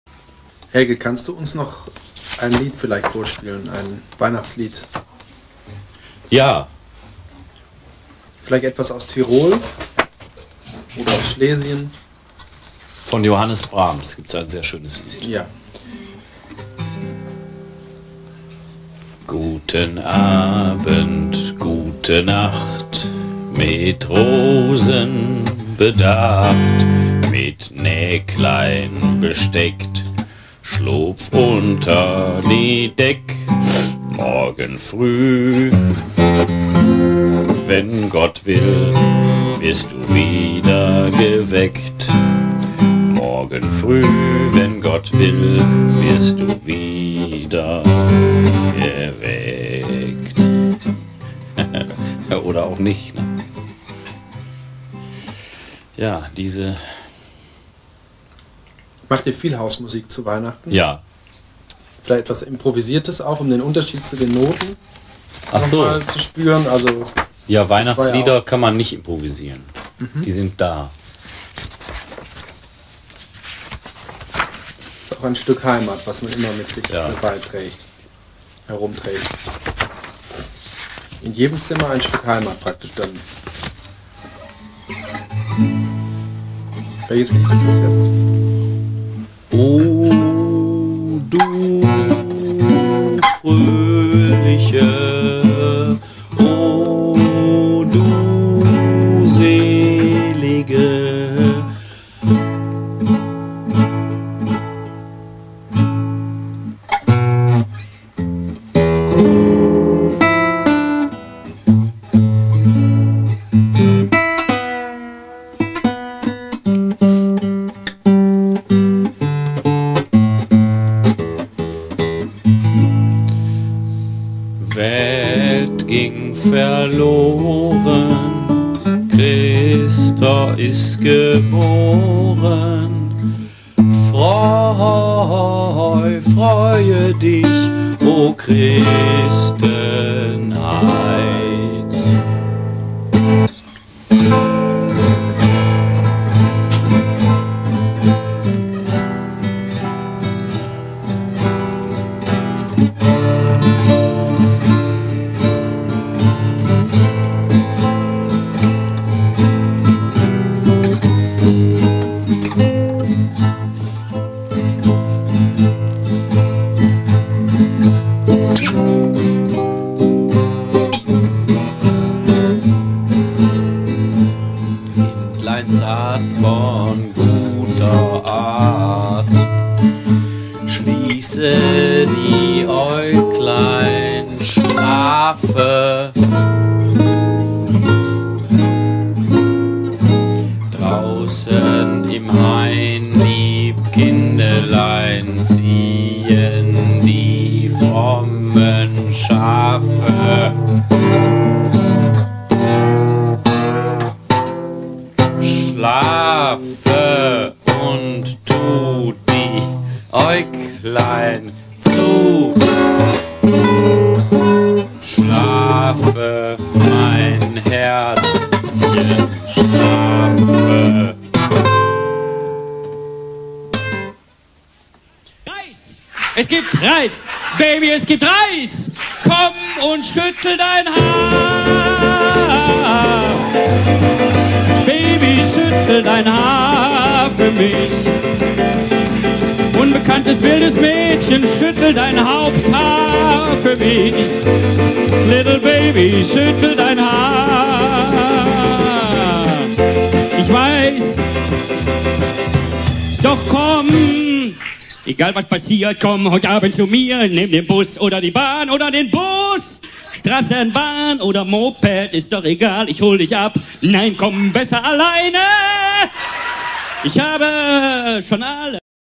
Helge Schneider singt Weihnachtslieder (mit Christoph Schlingensief) 3/3
Helge Schneider interpretiert Weihnachtslieder (die sind da!).